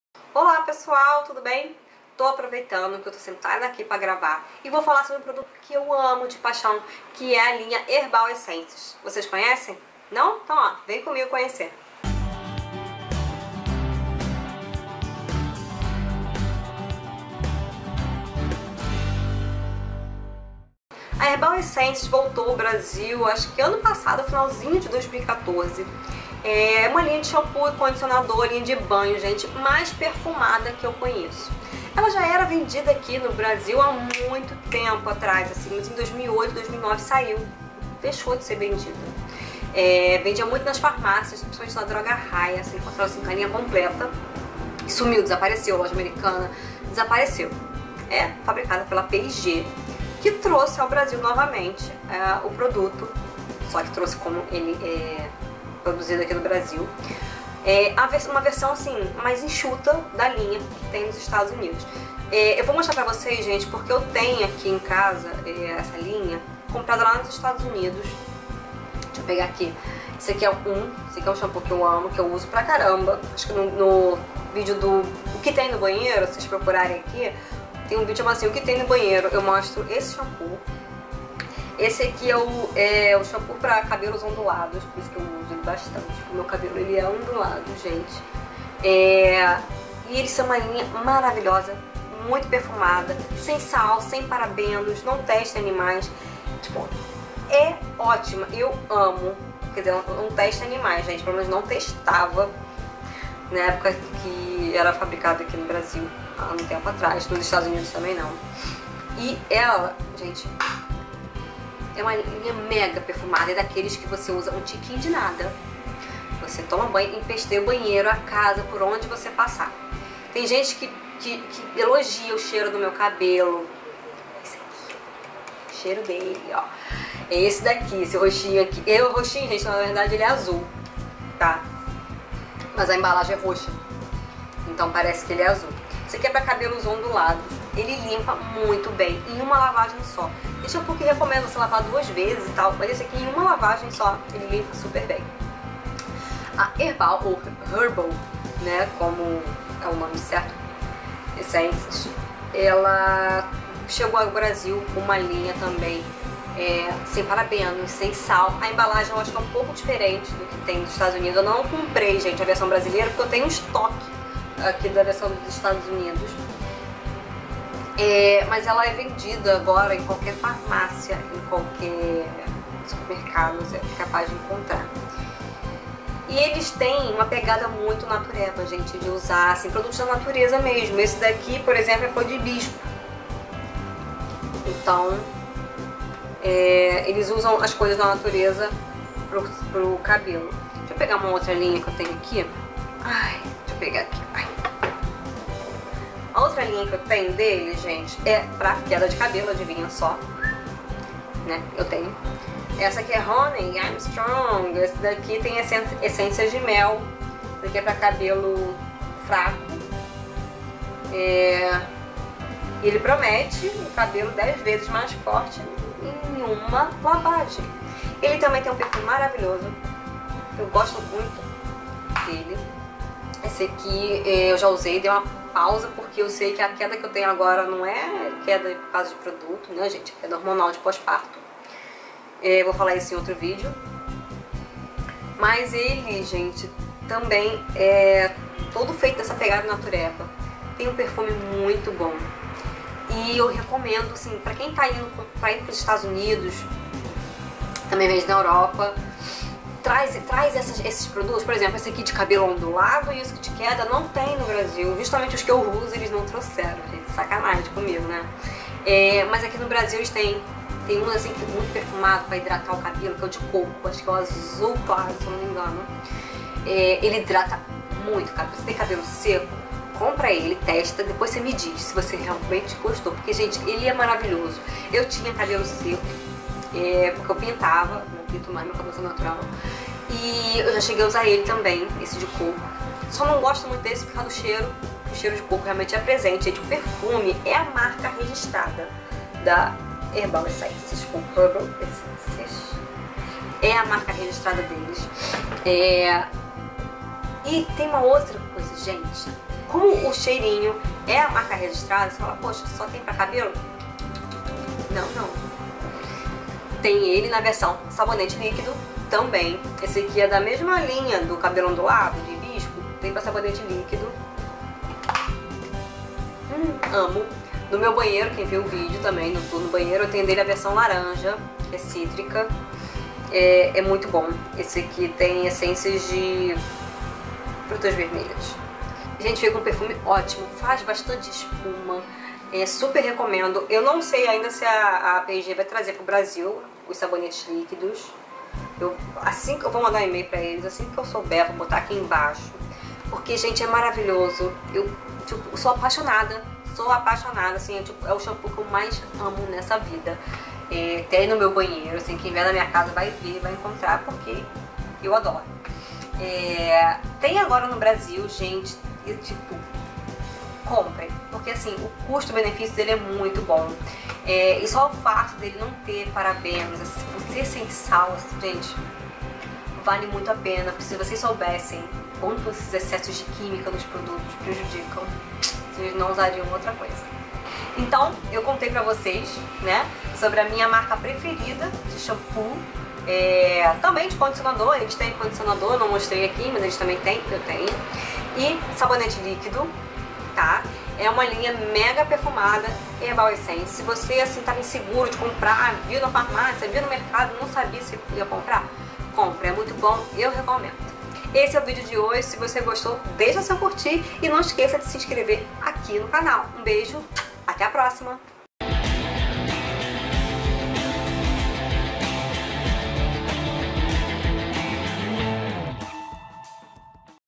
Sobre Herbal Essences
Nesse vídeo eu conto sobre a marca de shampoos que mais gosto e o porquê!